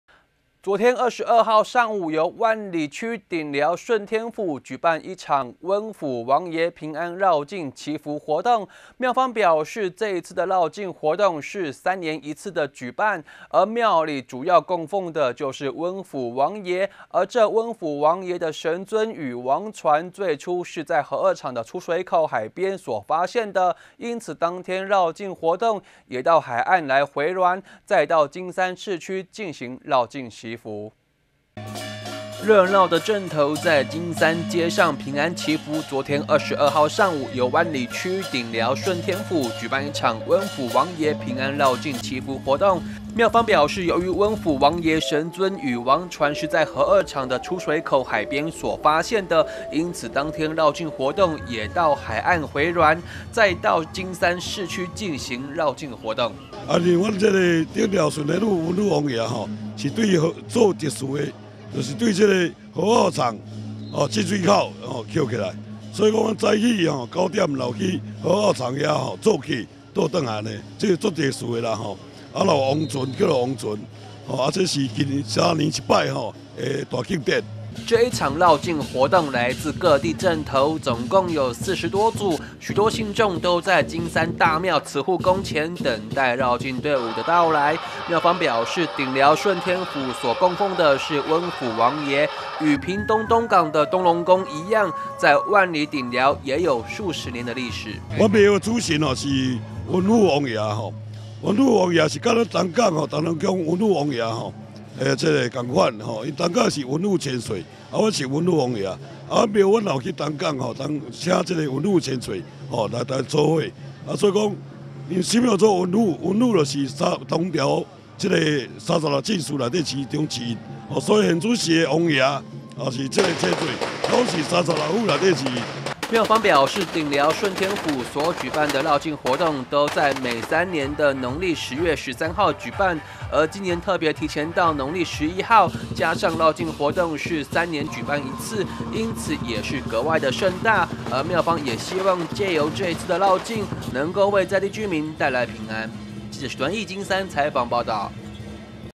1041123紅樹林有線新聞 頂寮順天府回鑾遶境 陣頭熱鬧場面盛大
昨天22號上午，由萬里區頂寮順天府，舉辦一場溫府王爺平安遶境祈福活動，廟方表示，遶境活動是三年舉辦一次，而廟裡主要供奉的是溫府王爺，這溫府王爺神尊與王船，最初是在核二廠的出水口海邊所發現，因此當天遶境活動，也到海岸回鑾，再到金山市區進行遶境活動。